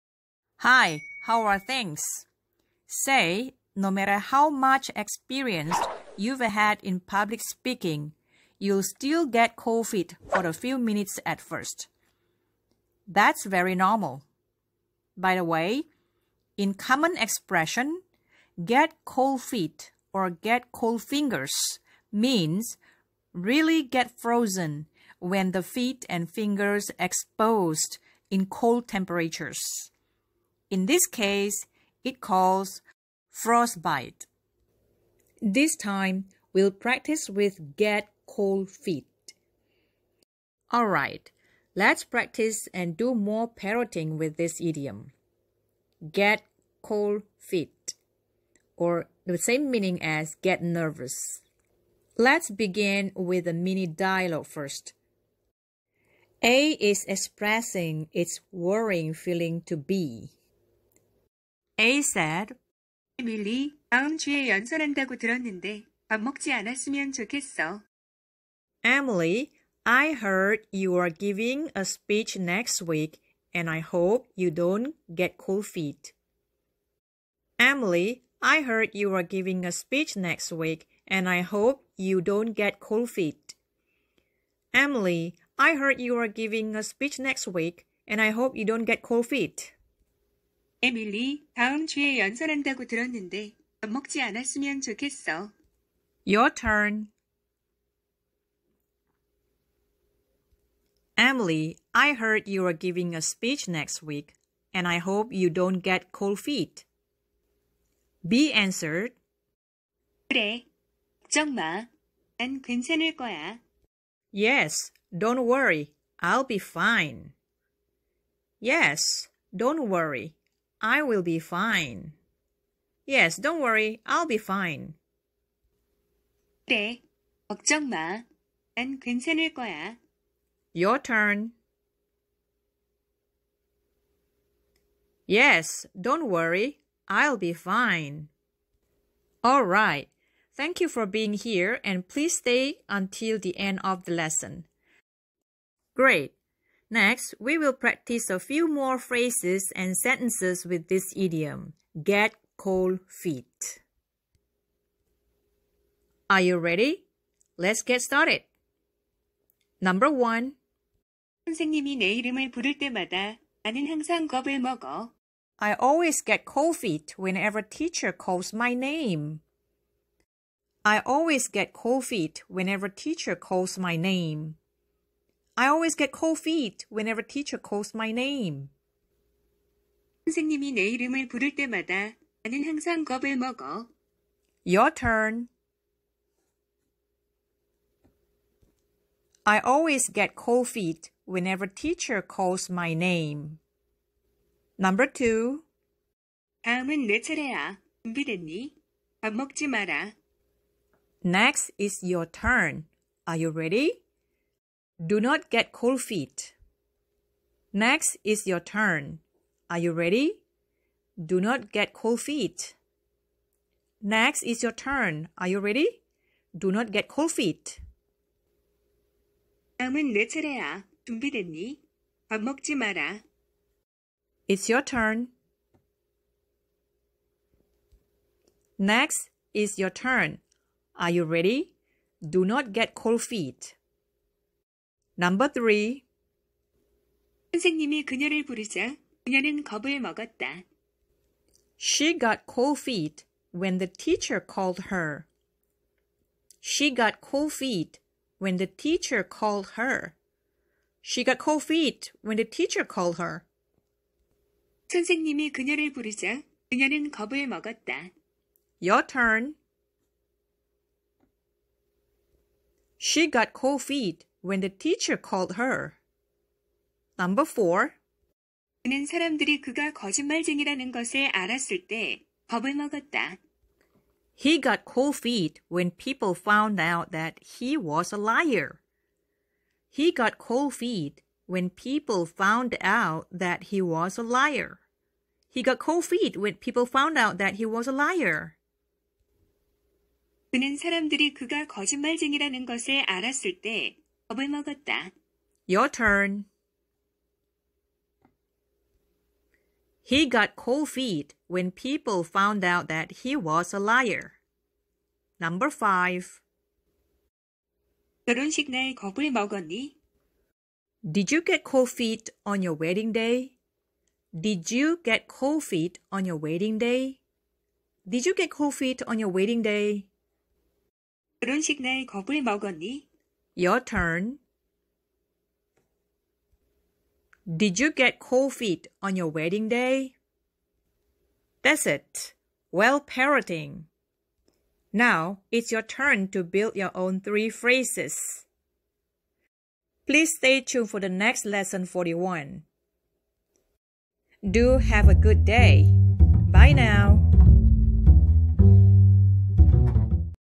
Let's Parroting 40: Get cold feet/ 떨린다, 긴장하다, 겁이난다 ~
Let's Parroting
 어려움 영어표현 쉽게 연습해요.
Learning English Expressions Video used to build for my Korean students at all ages at first But now I'm welcoming to everybody as well 🤗

Let's Parroting 40.
Hi! How are things?

Say~ no matter how much experienced you've had in public speaking, you'll still get cold feet for a few minutes. That's very normal.
아무리 경험 많은 사람들 앞에서 연설을 했어도 몇 분 동안은 냉담해질 것이다. 그건 지극히 정상이다.

By the way in common expression, "get cold feet or get cold fingers" means really get frozen when the feet and fingers exposed in cold temperatures. In this case it calls frostbite 동상걸린다.
그런데 흔히 말하는 'get cold feet'는 추운 날씨에 발과 손가락이 노출되면 정말 얼어버린다는 뜻이다. 이 경우 영어로:  frostbite 동상이라고 부른다.

This time we'll practice with "get cold feet."
이번에는 'get cold feet'로 연습할 것이다.
Alright, let's practice and do more parroting with this idiom:
Get cold feet = get nervous!
떨린다, 긴장하다, 겁이난다

좋아요, 이 관용어로 좀 더 연습하고 파로잉을 해보자.